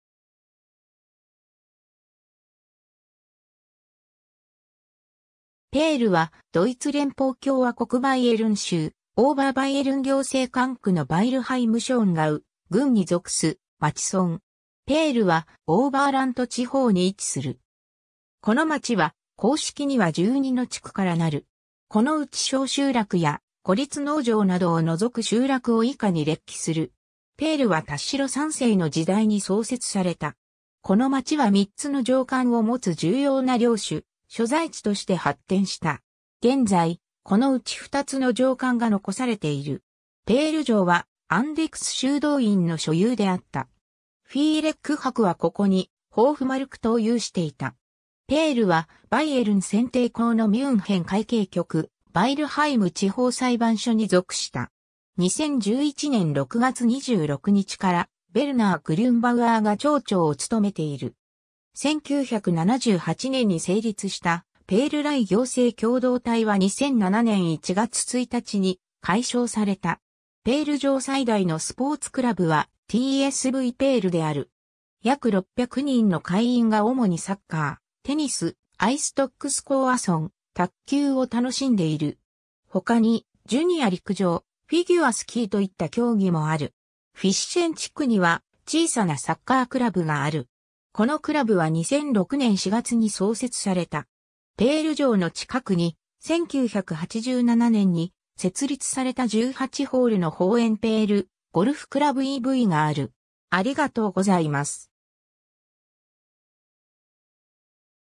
ペール
ヴァイルハイム＝ショーンガウ郡
ペール (ドイツ語: Pähl) は、ドイツ連邦共和国バイエルン州オーバーバイエルン行政管区のヴァイルハイム＝ショーンガウ郡に属す町村（以下、本項では便宜上「町」と既述する）。
 ペールはオーバーラント地方に位置する。
 この町は、公式には 12の地区 (Ort) からなる。
 ペールはタッシロ3世の時代に創設された。
この町は3つの城館を持つ重要な領主所在地として発展した。
現在、このうち2つの城館が遺されている。
ペール城（ホッホシュロス）はアンデクス修道院の所有であった。
フィーレック伯はここにホーフマルクトを有していた。
ペールは、バイエルン選帝侯のミュンヘン会計局、ヴァイルハイム地方裁判所に属した。
 2011年6月26日からヴェルナー・グリュンバウアー (Unabhängige Bürgerliste für Pähl und Fischen UBL) が町長を務めている。
 1978年に成立したペール＝ライスティング行政共同体は2007年1月1日に解消された。
 ペール城 最大のスポーツクラブはTSVペールである。
約600人の会員が主にサッカー、テニス、Eisstockschießen、卓球を楽しんでいる。
他にジュニア陸上、フィギュアスキーといった競技もある。
フィッシェン地区には小さなサッカークラブがある。
このクラブは2006年4月に創設された。
 ペール城の近くに、1987年に設立された18ホールのホーエンペール・ゴルフクラブe.V. がある。